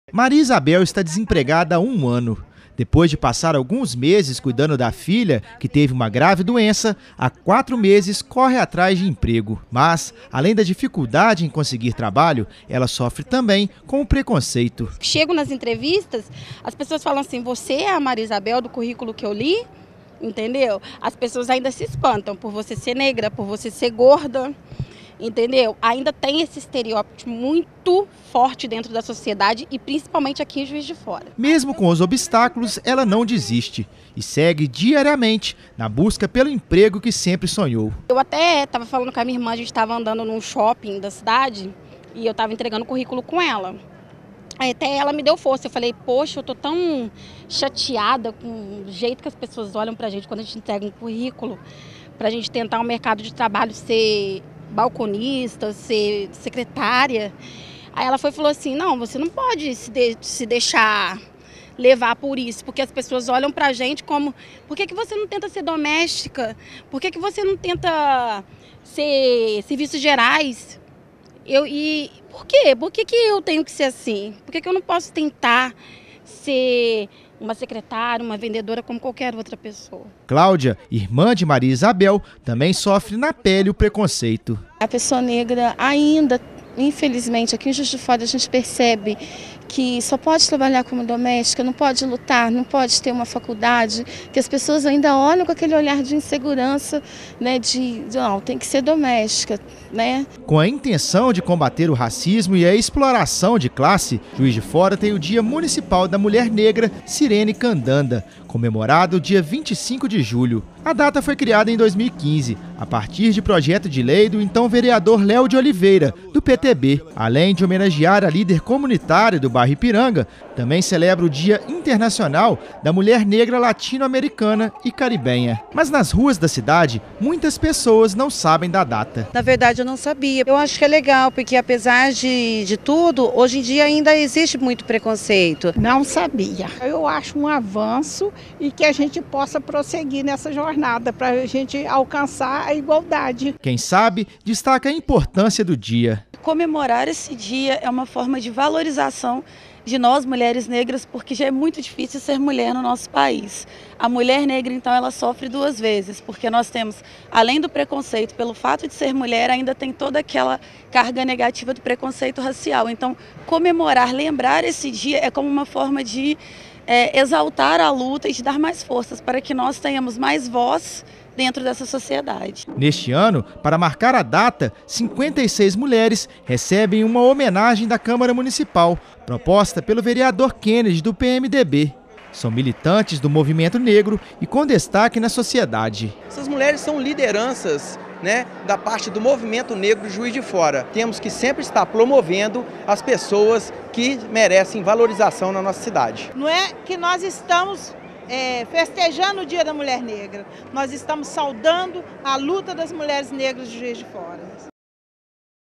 Dia Municipal da Mulher Negra é lembrado pela Câmara Municipal
Mais de 50 mulheres líderes do Movimento Negro e com destaque na sociedade foram homenageadas pelo Legislativo, através de Moção de Aplauso do vereador Kennedy (PMDB). A data foi criada em 2015, após Projeto de Lei. Apesar da importância do dia, as negras ainda convivem com preconceito e falta de reconhecimento.